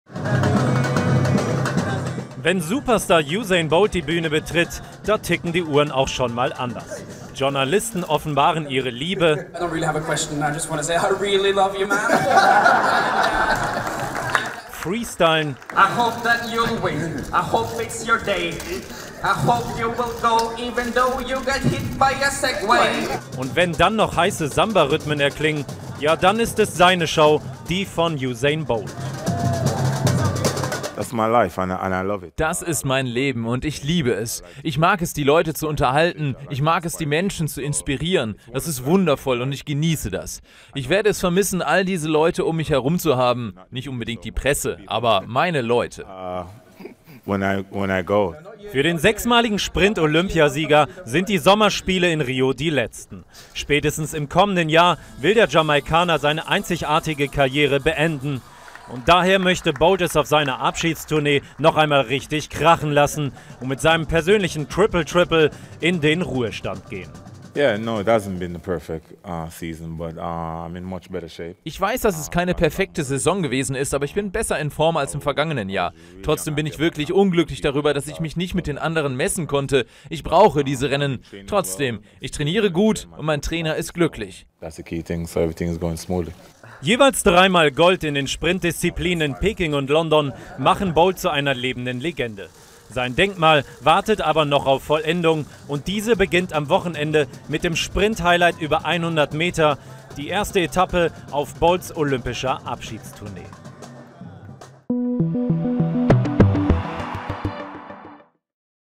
Usain Bolts Samba-Zirkus in Rio
Superstar Usain Bolt lässt es bereits vor seinem ersten Wettkampf bei den Olympischen Spielen in Rio krachen, Samba mit brasilianischen Tänzerinnen inklusive.